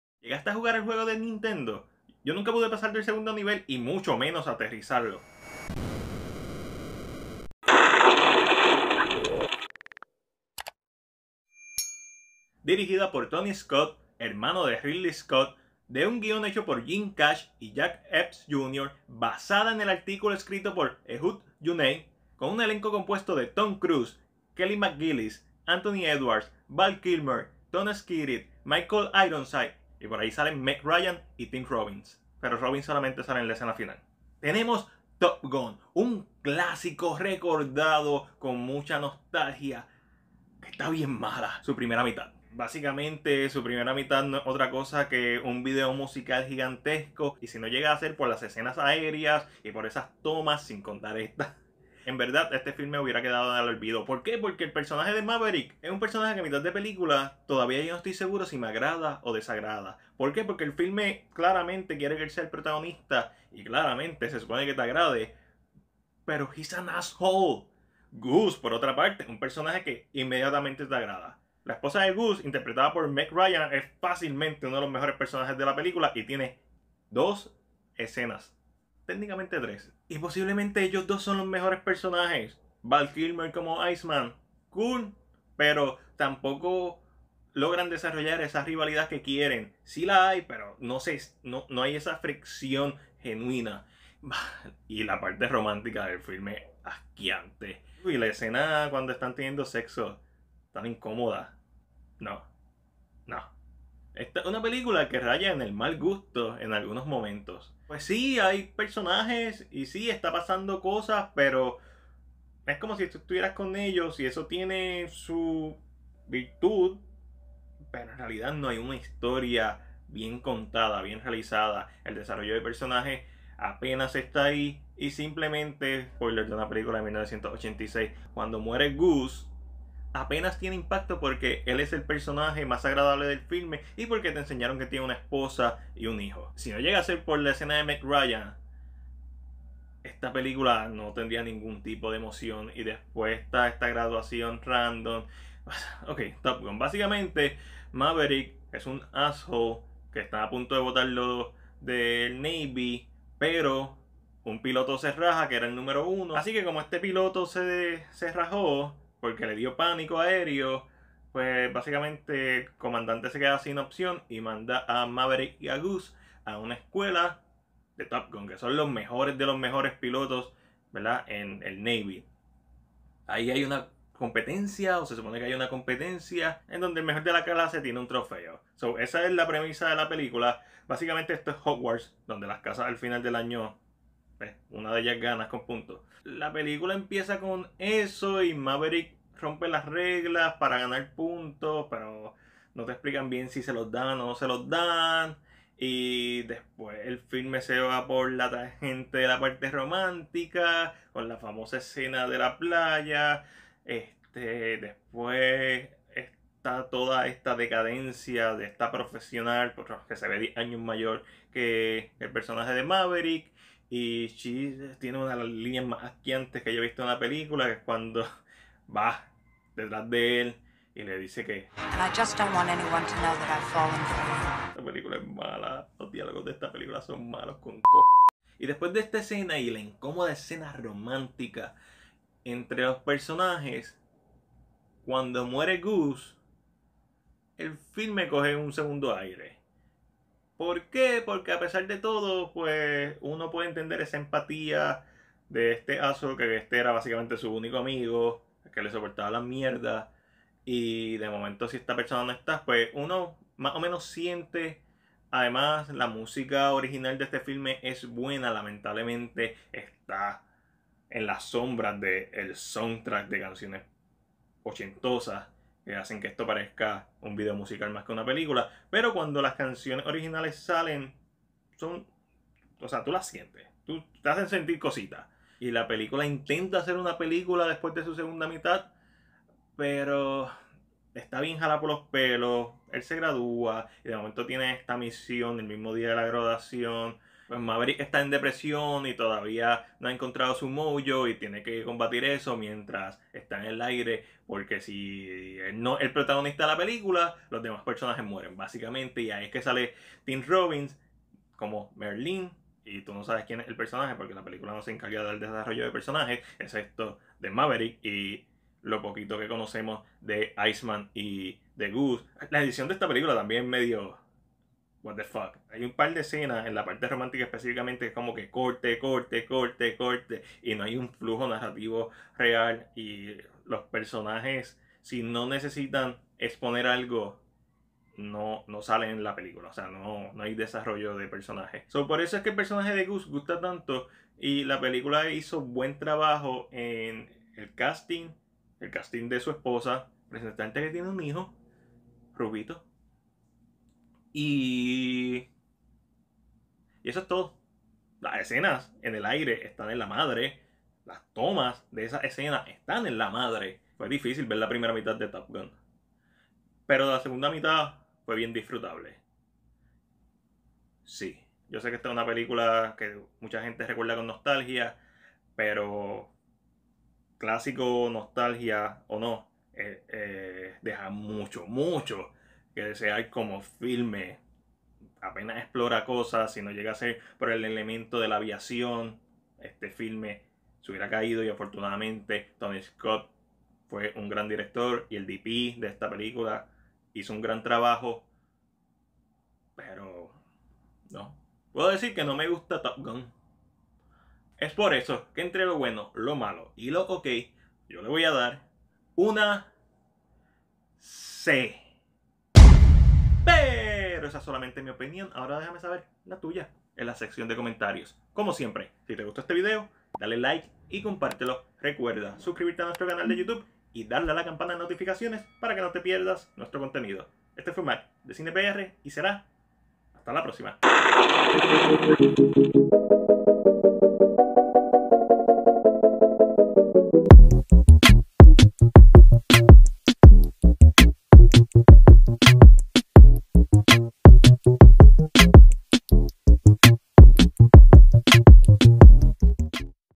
Top Gun (1986) ¿Tan buena como la recordamos?
I feel the need... de hacer esta reseña y revisitar este "clásico" de 1986.